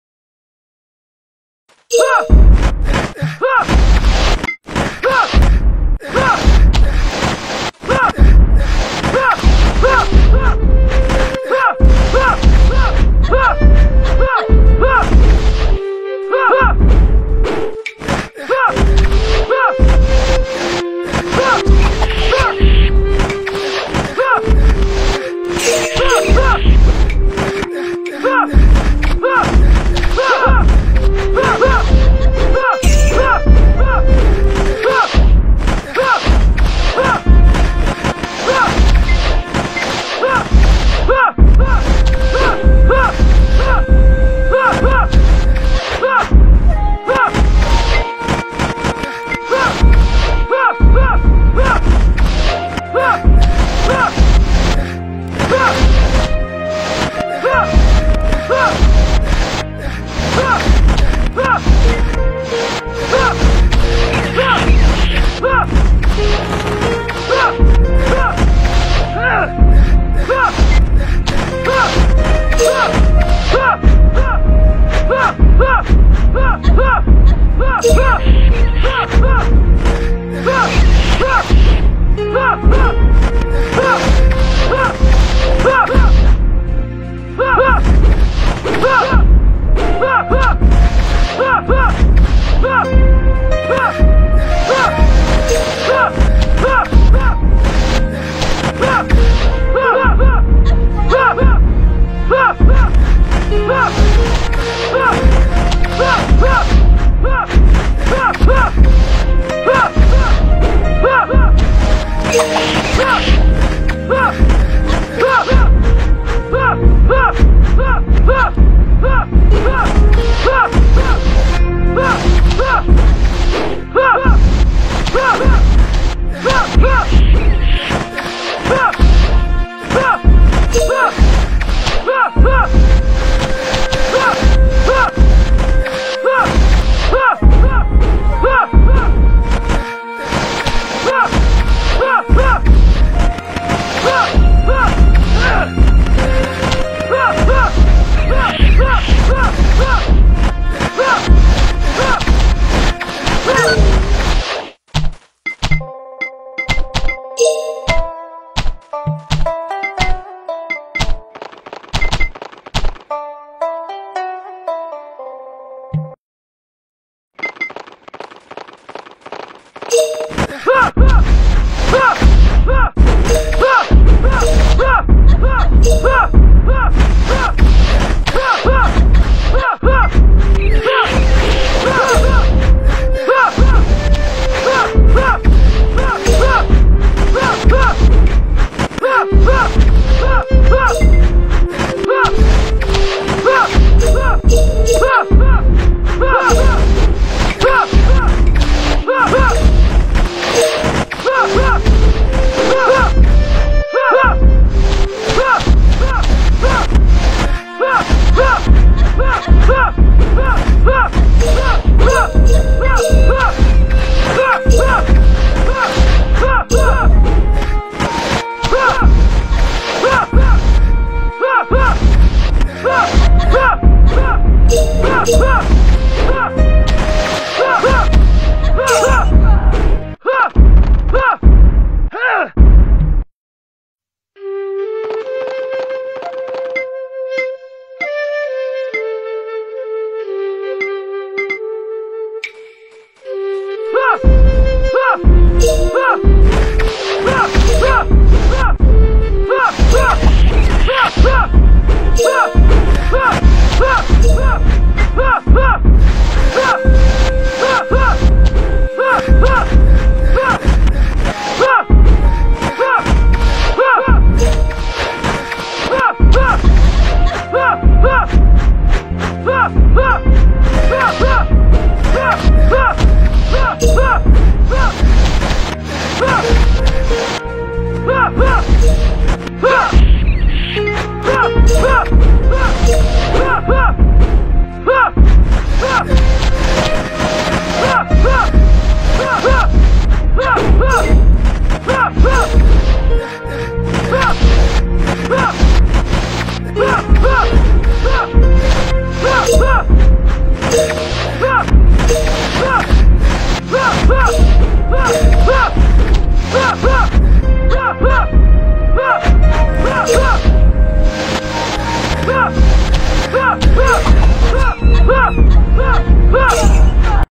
Đường Môn Nhiếp Hồn solo Thiên Vương Chùy - Trận đánh hay của cao thủ
Anh em có cách PK nào hay hơn có thể bình luận bên dưới nhé